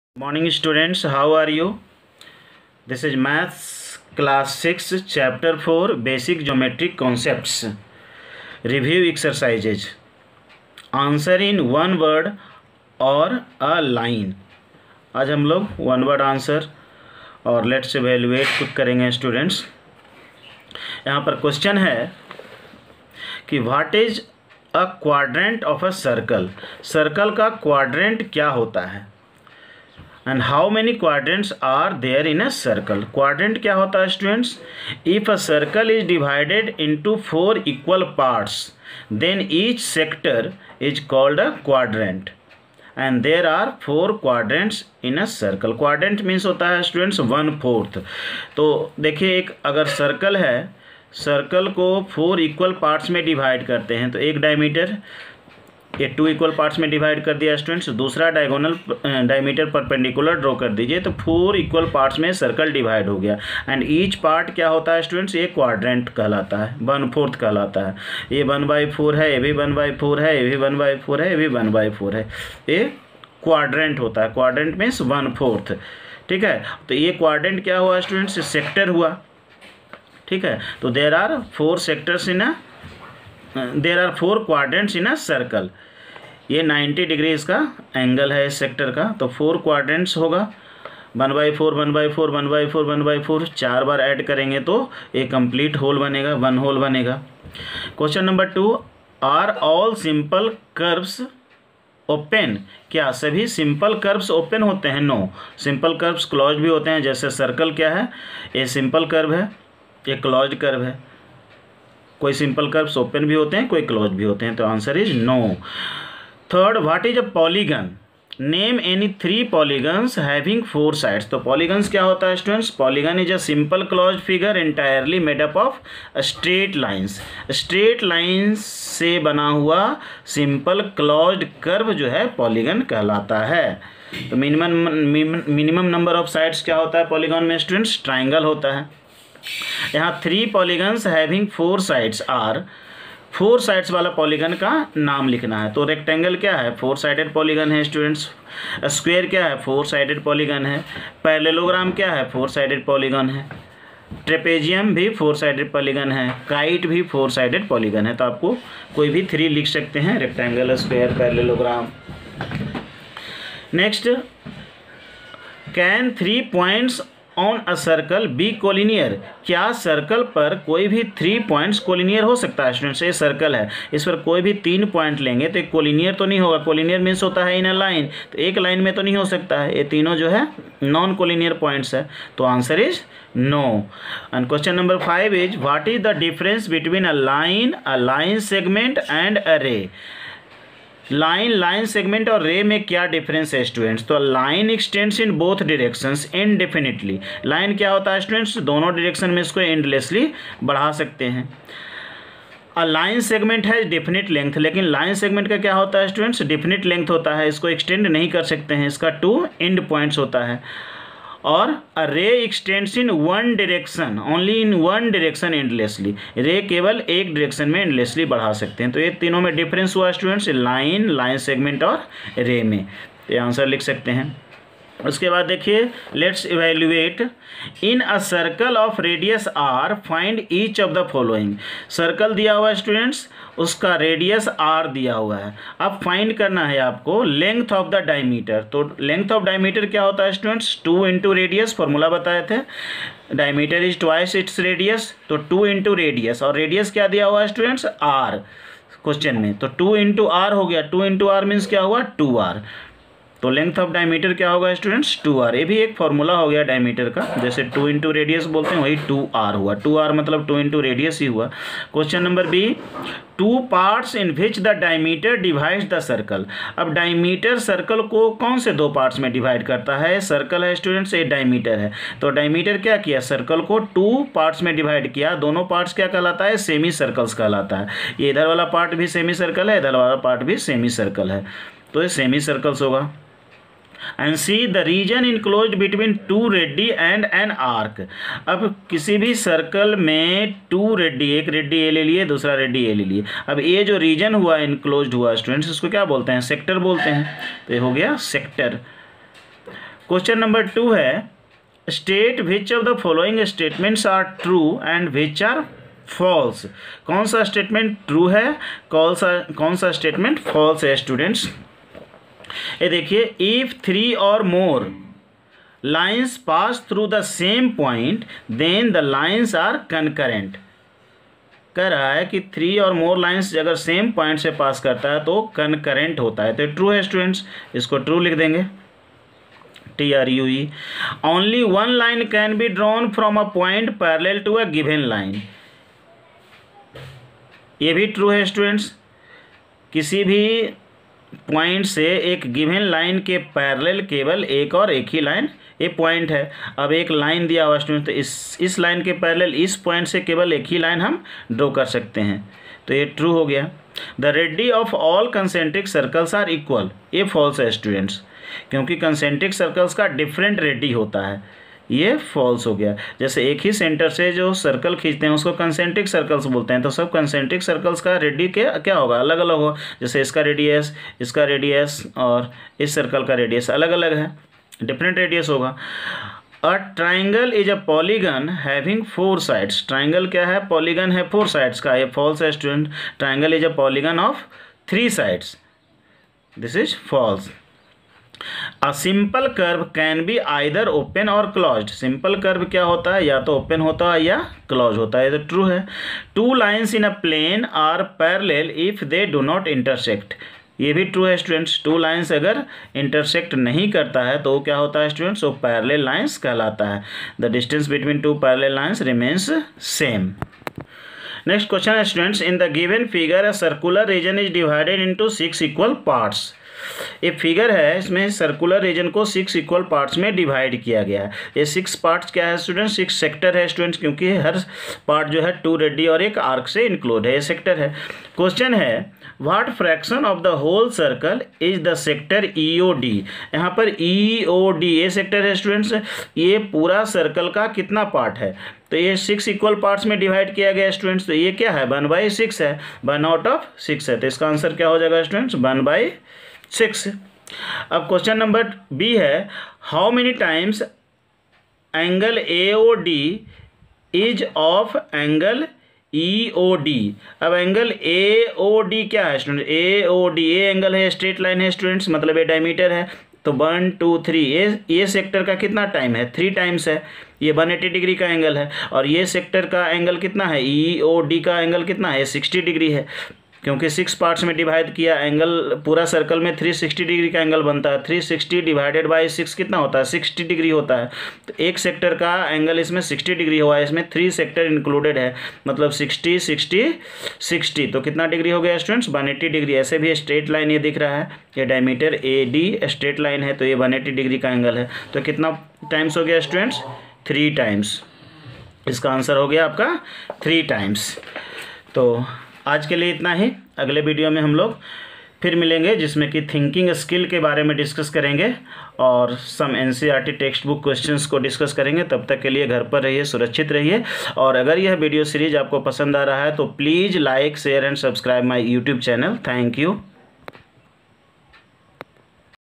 Part 14 | NCERT | MATHS | CLASS VI | CHAPTER 4 | Basic Geometric Concepts | Revision
Maths class VI Ch - 4 Basic Geometric Concepts have been reviewed through various types of questions. Must Watch MATHS Video Series by Barun Kumar Maths Teacher CPS Ratu Ranchi. Unique Style. Excellent techniques. Maths become fun.